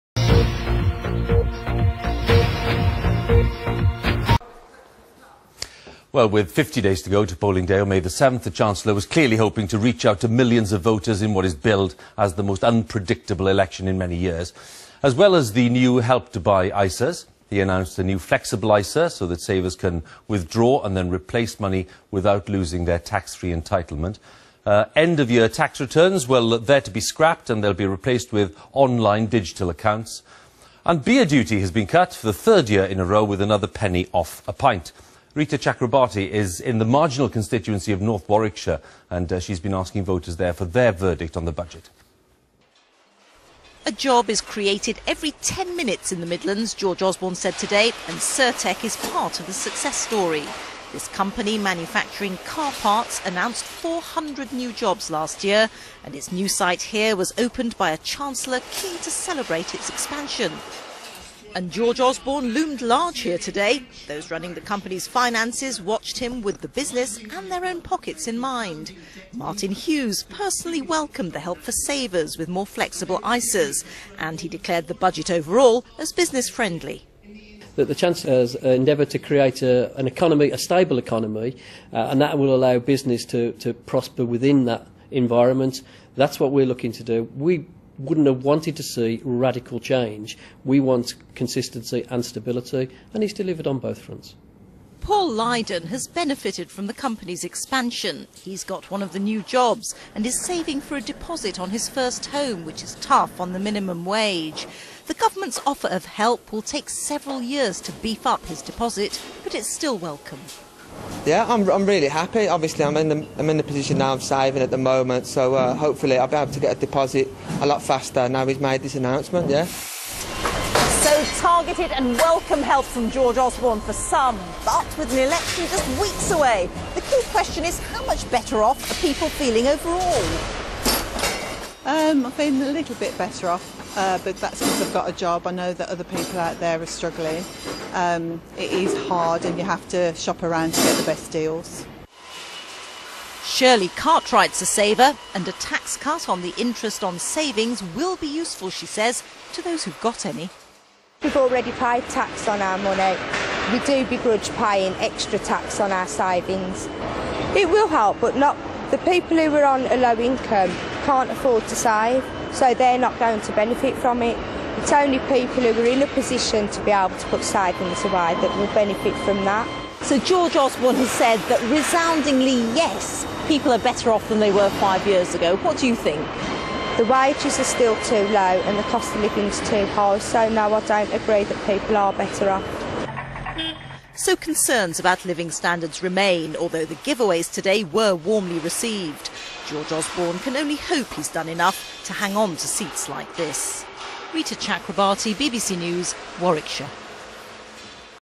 Sertec featured on BBC News 18/03/2015 22:10
When the BBC covered the Budget, where better place to film reaction to what was said than at Sertec's Wincaster House, officially opened by the Chancellor just 14 months earlier?!